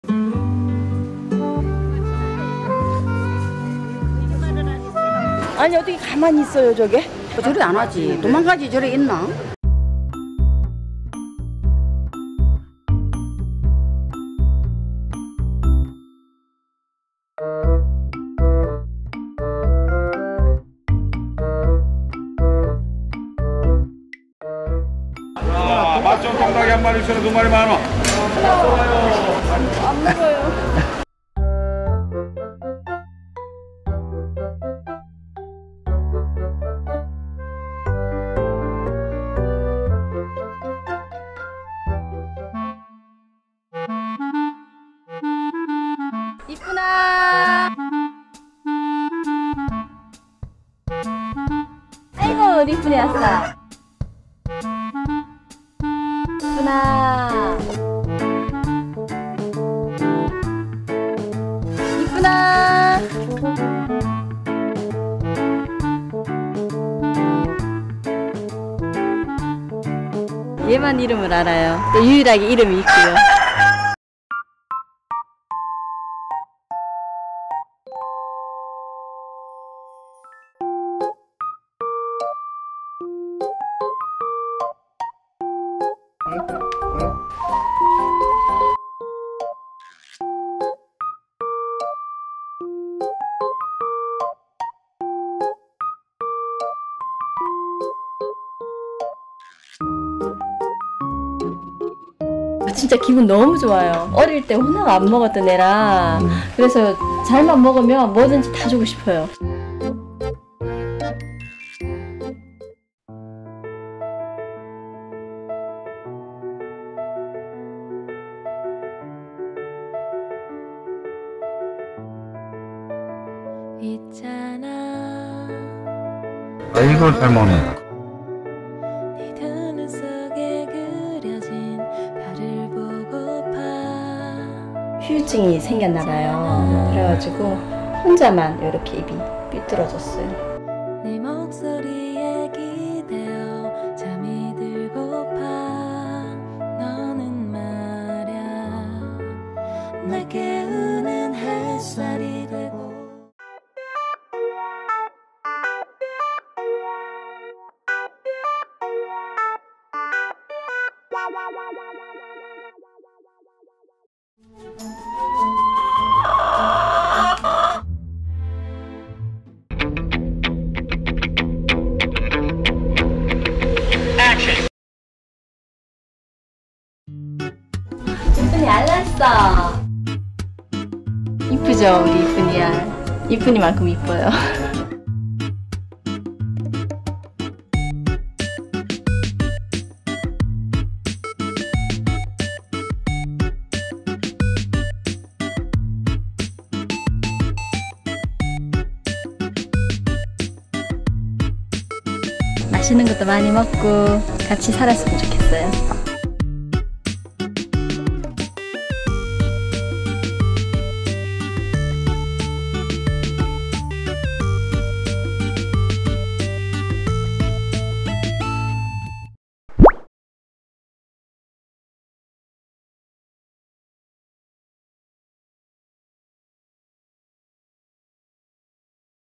Orphaned Chicken Acts Like A Dog To Her New Family | Kritter Klub
The angle couple adopted chicken hen Yippun who was abandoned by her family for being born with a crossed beak. Now Yippun has become a sidekick who follows their hooman parents, even to the market lol. Yet, one day, Yippun went missing inside the house.. What happened to her?